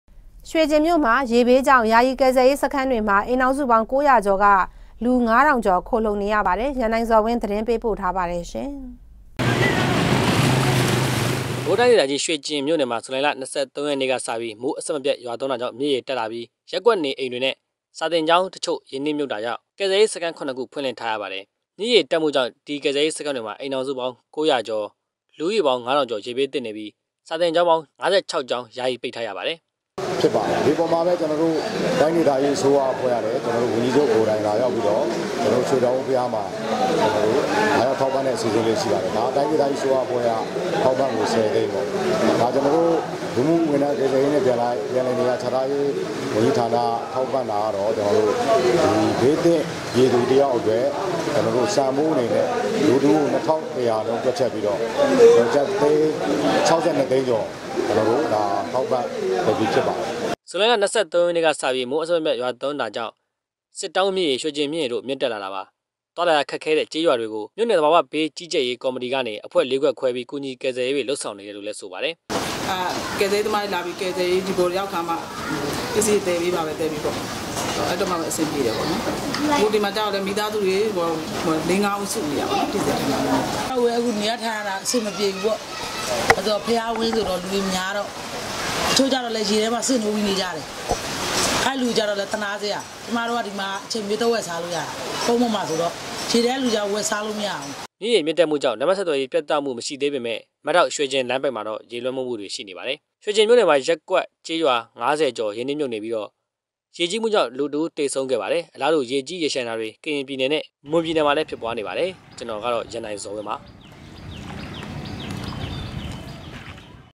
DVB - ေရေဘးေၾကာင့္ ယာယီကယ္ဆယ္ေရးစခန္းေတြမွာ လူ ၅၀၀၀ ေက်ာ္ ခိုလႈံ
ေရႊက်င္ျမိဳ့မွာ ေရေဘးေၾကာင့္  ယာယီကယ္ဆယ္ေရးစခန္းေတြမွာ အိမ္ေထာင္စုေပါင္း ၉၀၀ ေက်ာ္က လူ၅၀၀၀ေက်ာ္ခိုလႈံေနရပါတယ္။ ရန္ႏိုင္ေဇာ္ဝင္း တက္ဆက္ေပးမွာပါ။